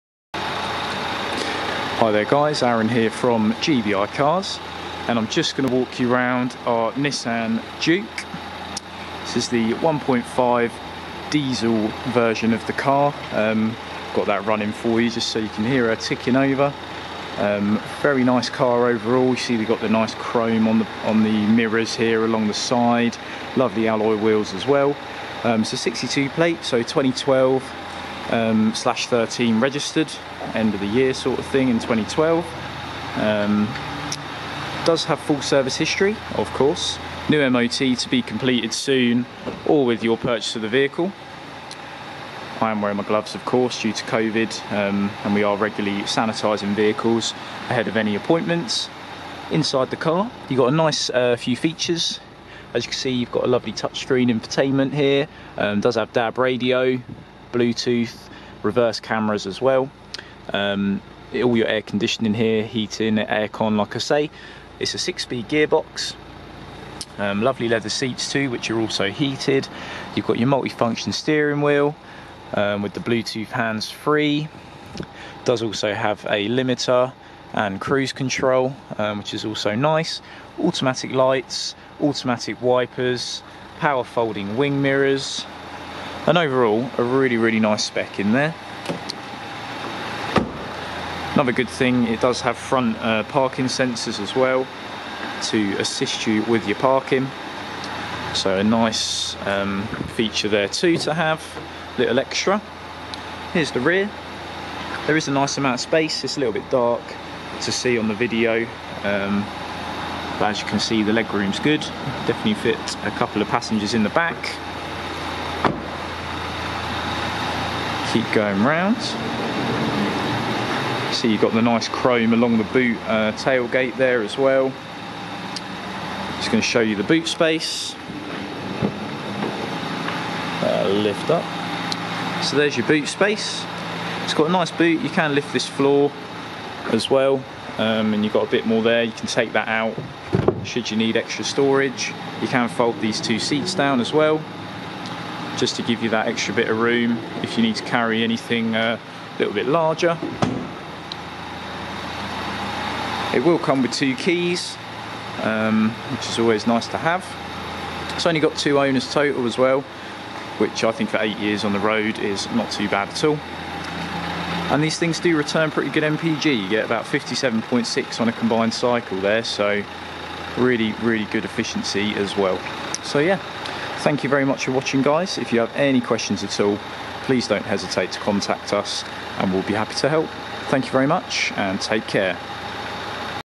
Nissan 1.5 dCi Tekna SUV 5dr Diesel Manual - LL62 VPP
Please enjoy our sales video for this Nissan Juke. Finance available, Free 3 months warranty (options to upgrade from £299), AA approved.